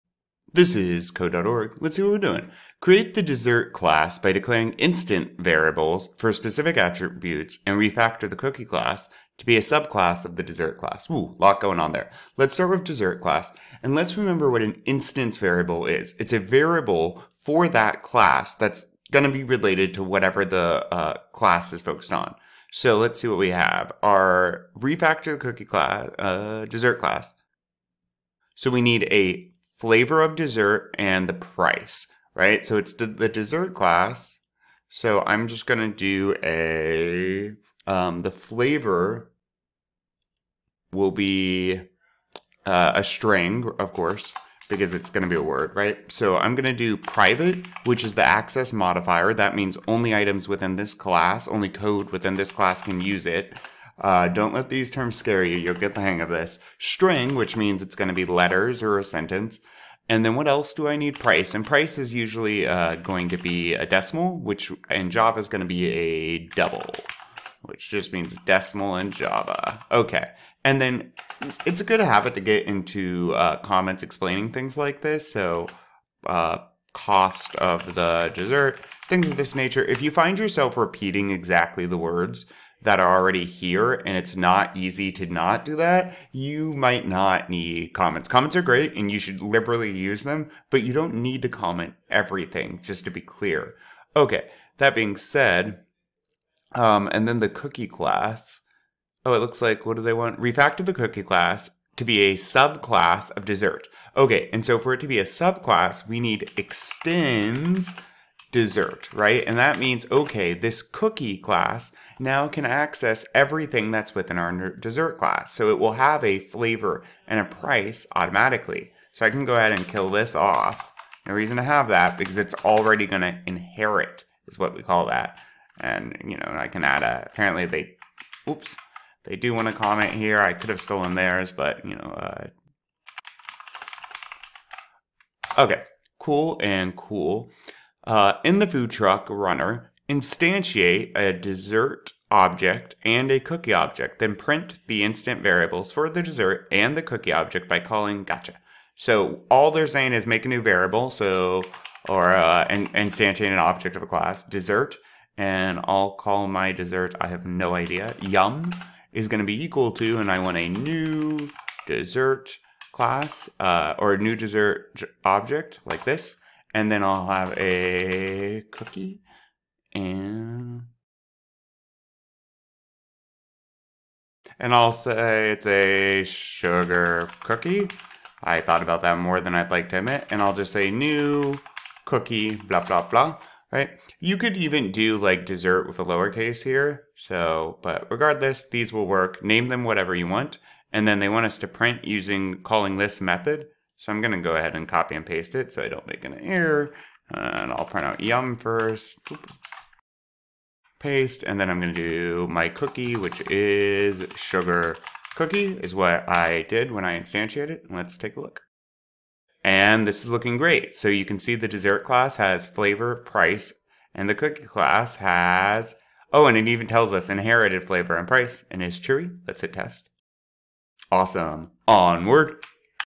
Code.org Lesson 1.9A Attributes | Tutorial with Answers | Unit 1 CSA
Class Structure and Design is unit 2 of Code.org's Computer Science A (CSA) course. The course is often used in AP Computer Science classrooms. Students utilize Java (programming language) to accomplish tasks by designing, writing, and testing computer programs. In this video, I walk through the step-by-step how to get the right answer for this puzzle.